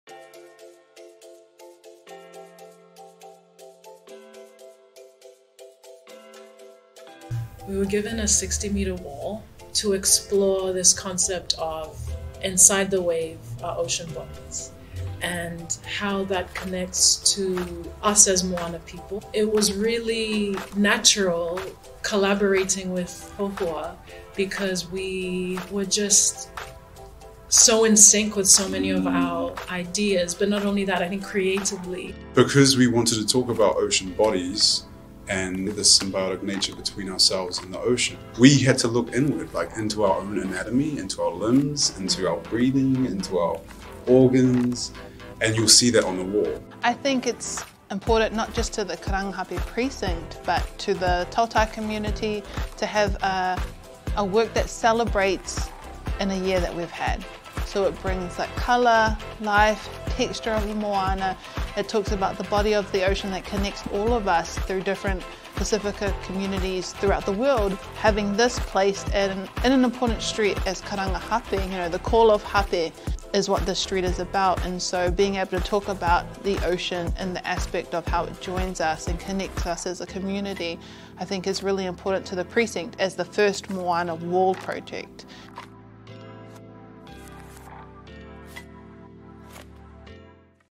MOANA WALL - Promo Video
Witness the connection between artists Hōhua Kurene and Natasha Ratuva and how that collaboration translated into an epic 60m site-specific work Inside the wave are ocean bodies, curated by Cora-Allan Wickliffe.
“We had to look inward, into our anatomy, into our limbs, into our breathing, into our organs and you’ll see that on the wall.”
- Hōhua Kurene
We cannot wait to share this new space and incredible exhibition with you all!
MOANA WALL Exhibition Opening
Friday 4 December, 6pm-Late
East Street, Auckland CBD
FREE EVENT
📹: No Six